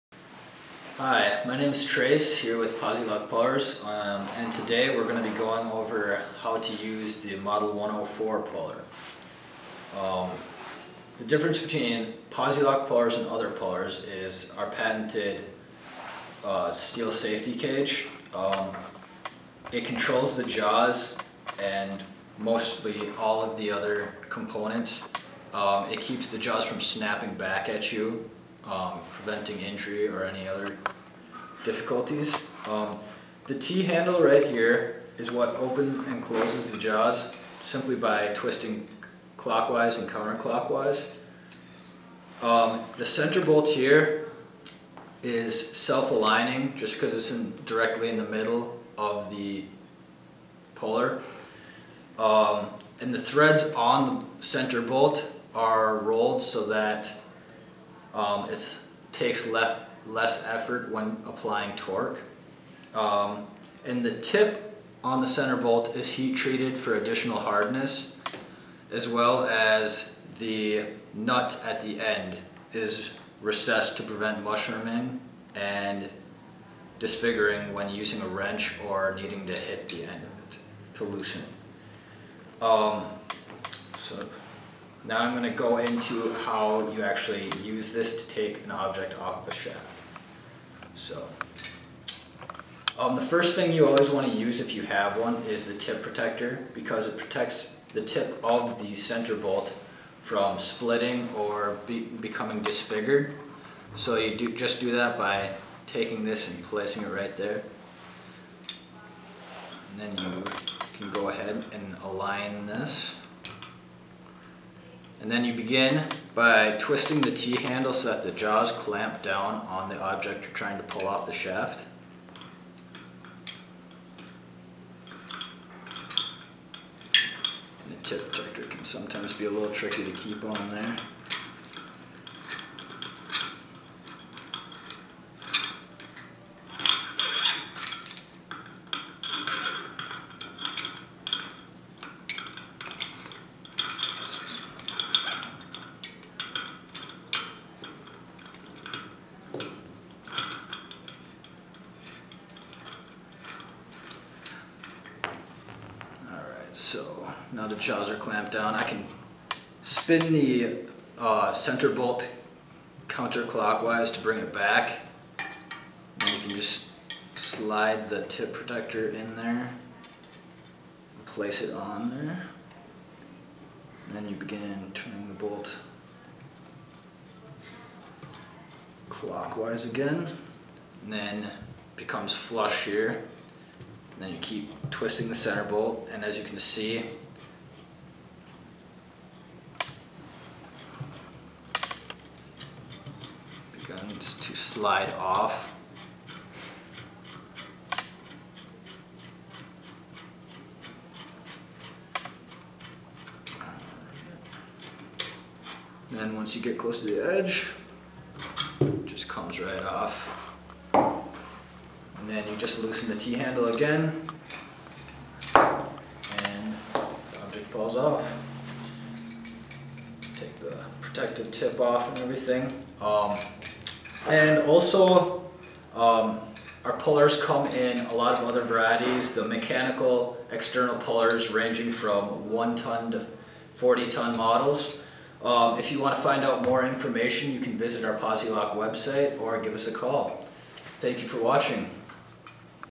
Posi Lock Mechanical Puller Demo
Our intern, Trace, was put to the test to do a demo video of our mechanical pullers.  He did great for being put on the spot. Thanks Trace!